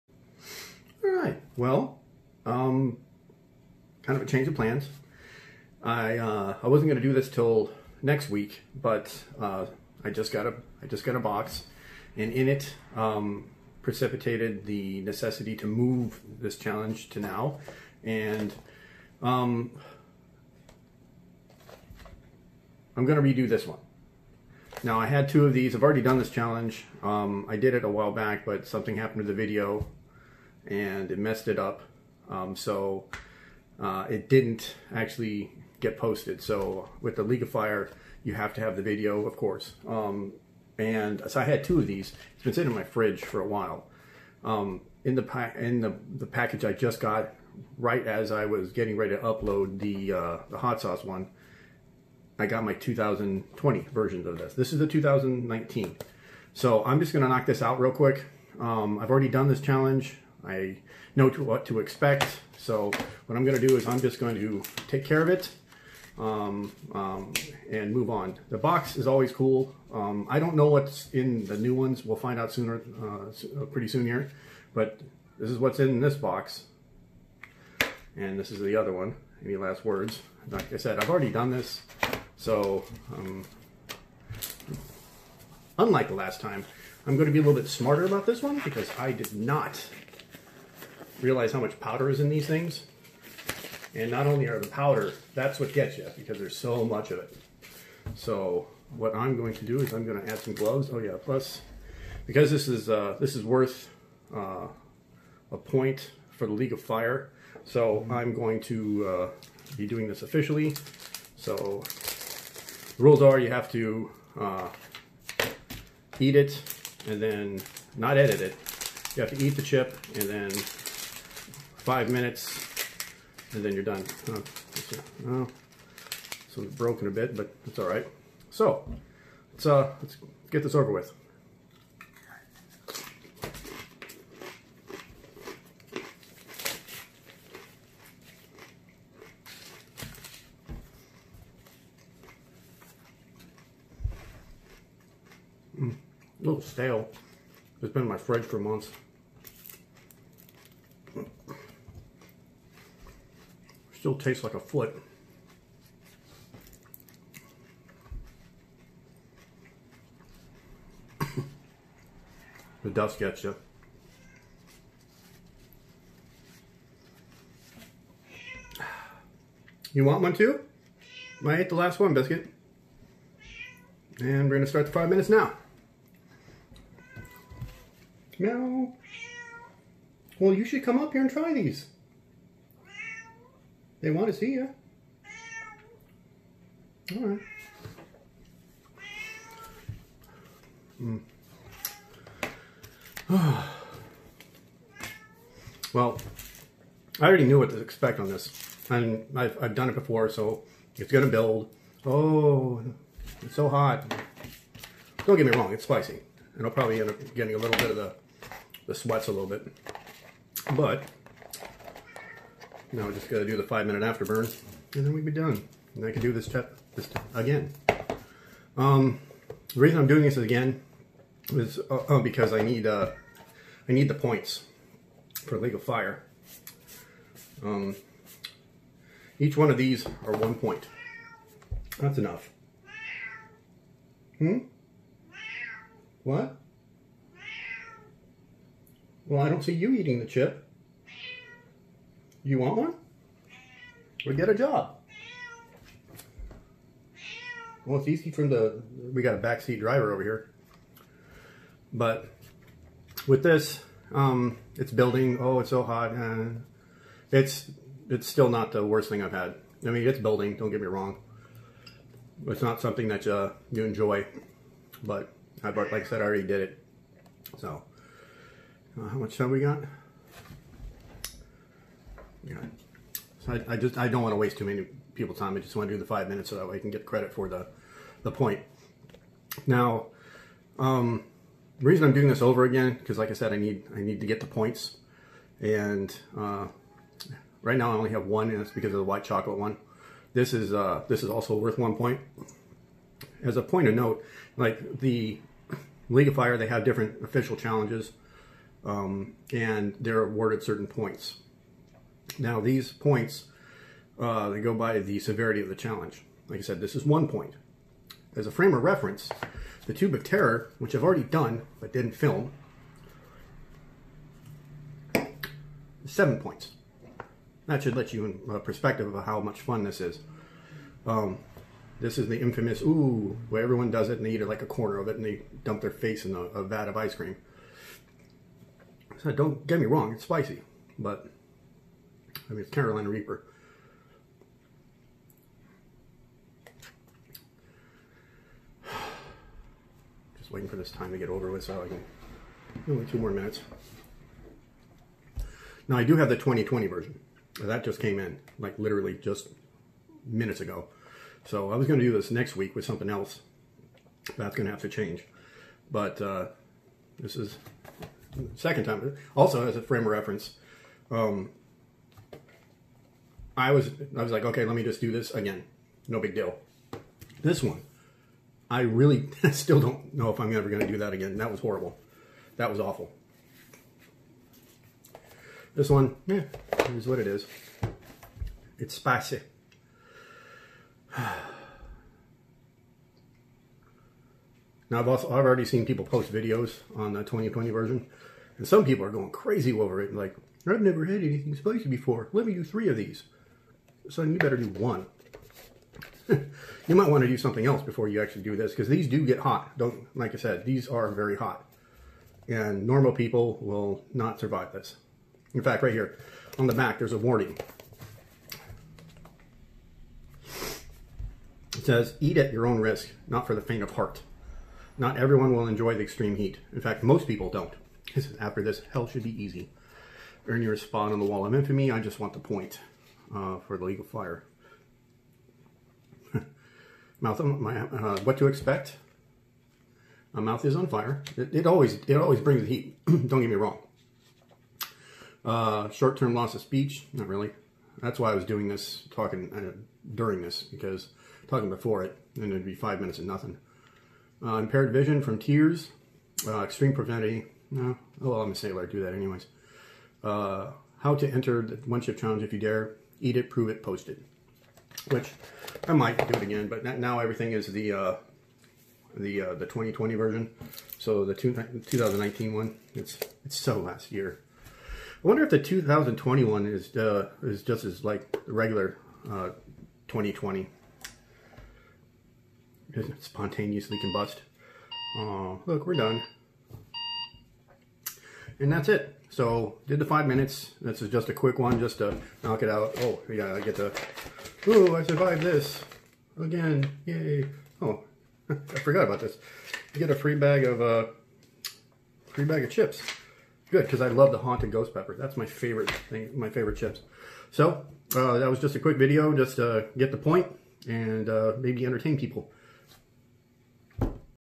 2019 Paqui One Chip Challenge (round 2)
First ever attempt at uploading a challenge didn't go well for me. So, I am re-doing this one over again in prep for the 2020 version.